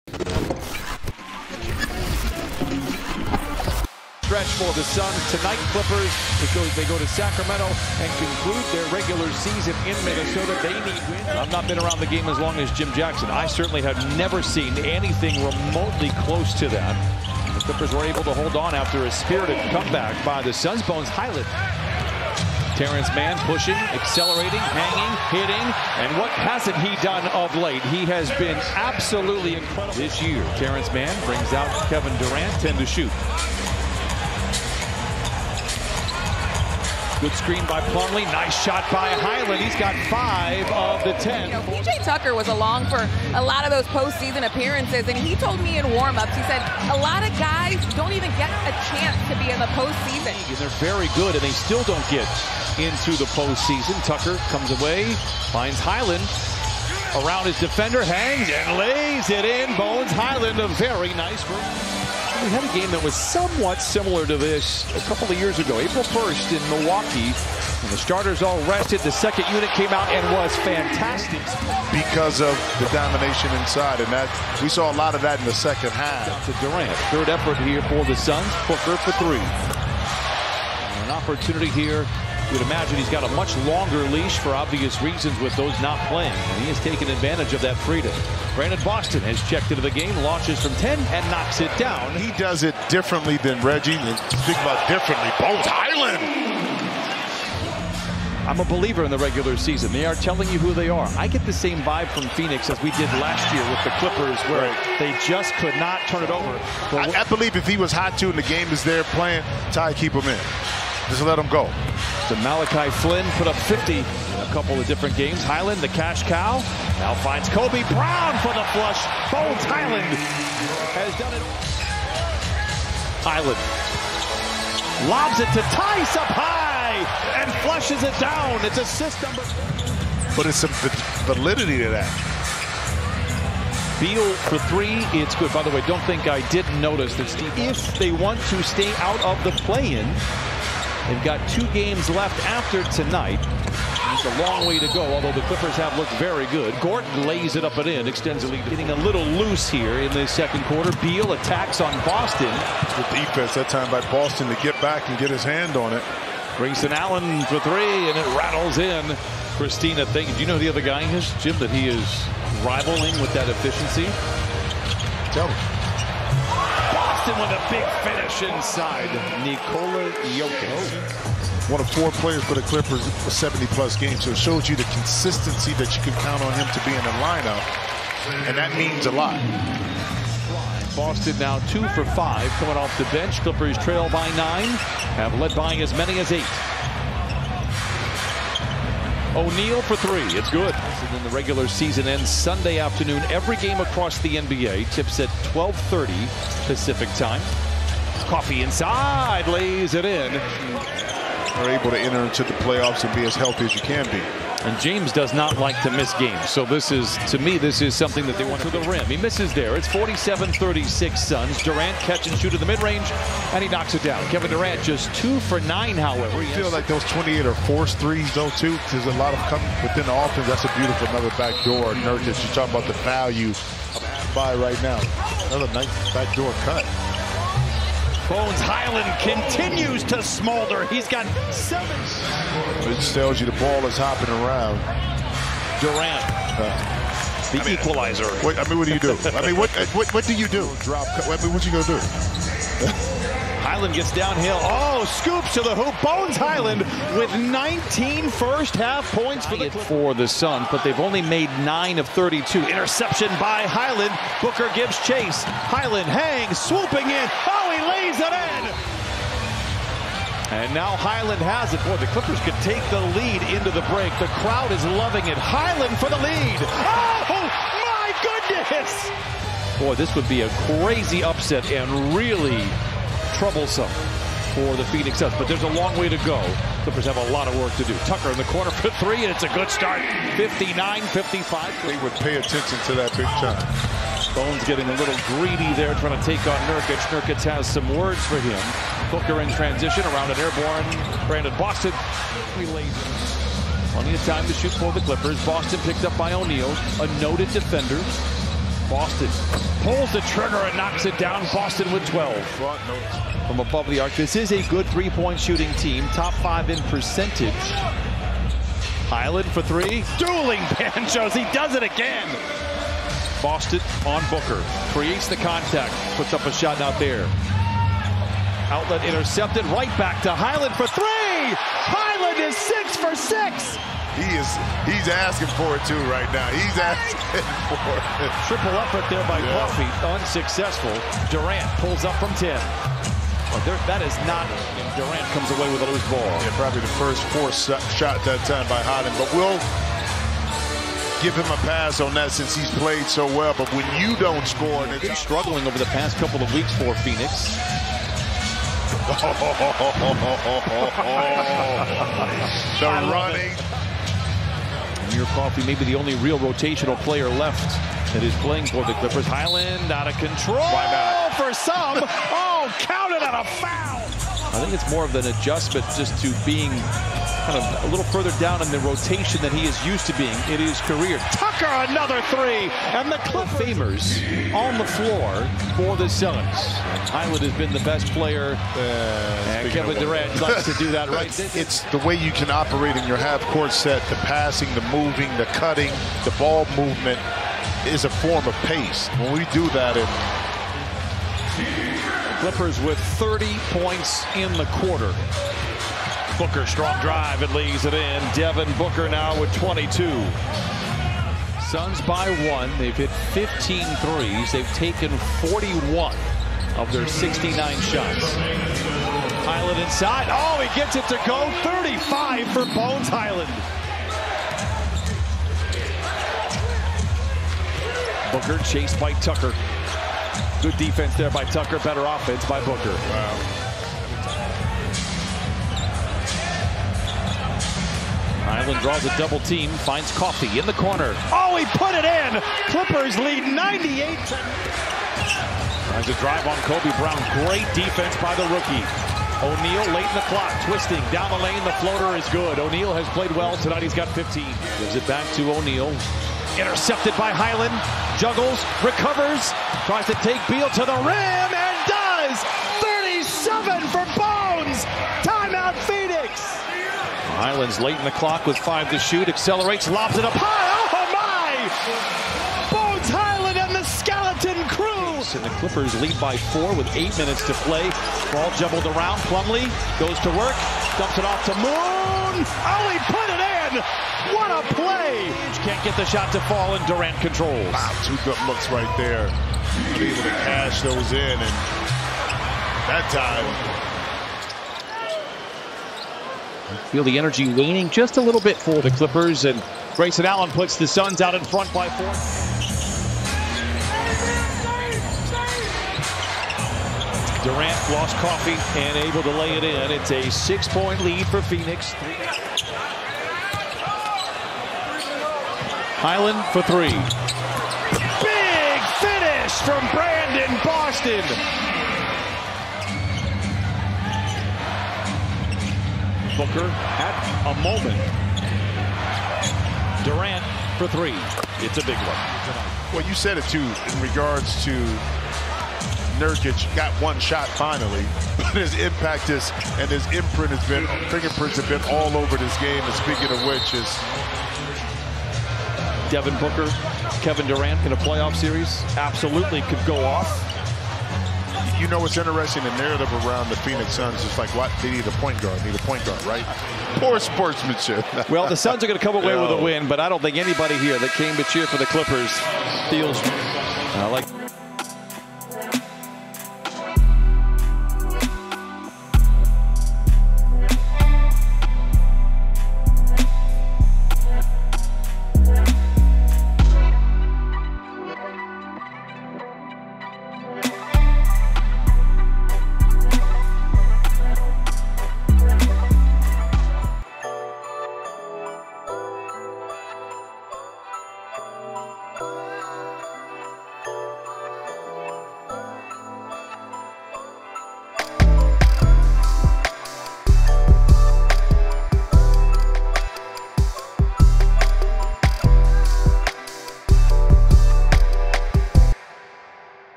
Phoenix Suns vs LA Clippers - Full Game Highlights | April 10, 2023-24 NBA Season
Place your picks with Underdog Fantasy!